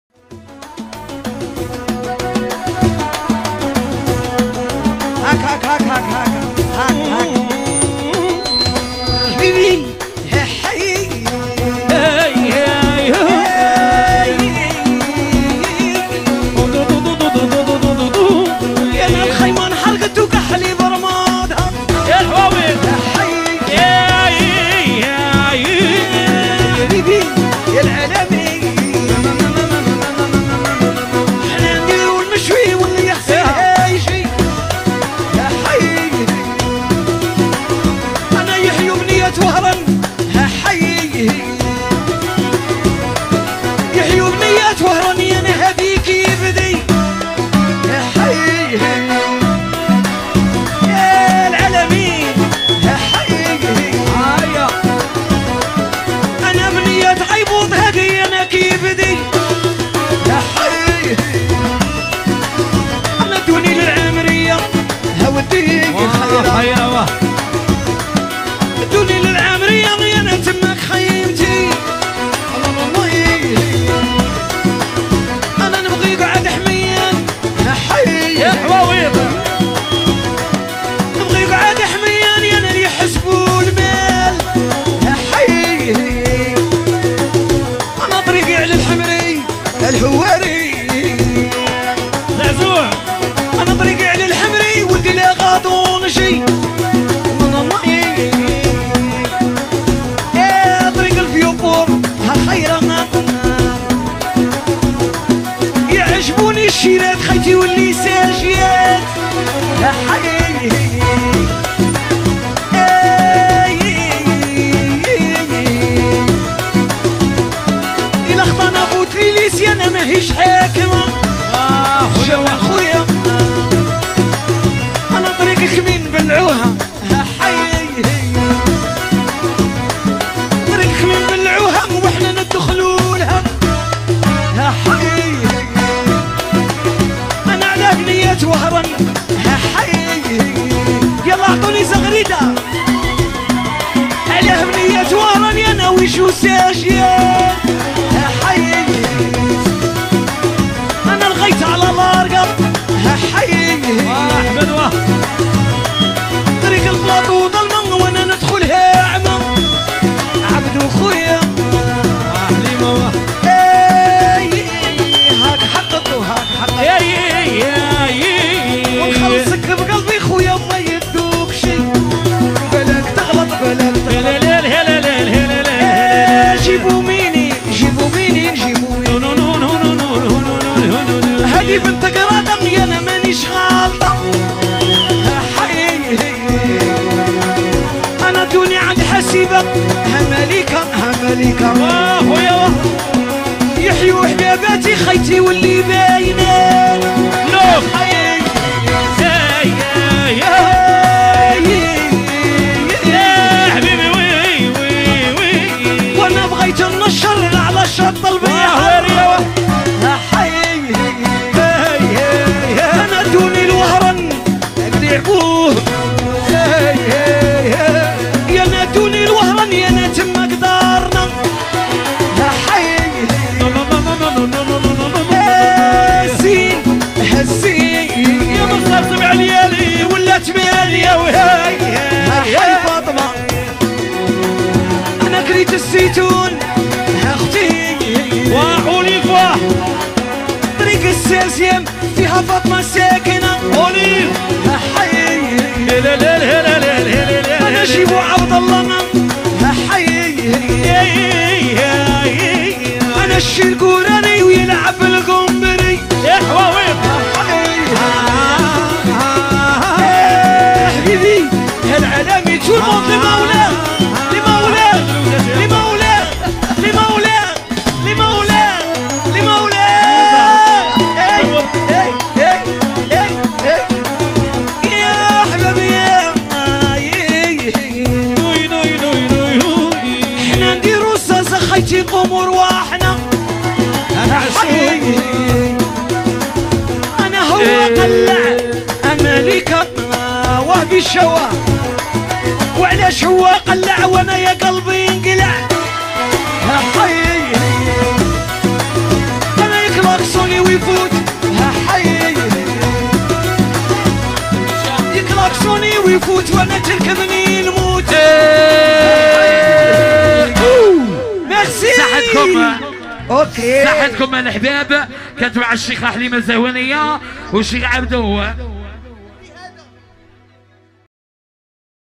zahouania dou chikh abdou avec wahidovitch maesro 2020 live mode mariage 100% medahette
tres bon live zahouania et chikh abdou avec wahidovitch maestro #2020 mariage 100%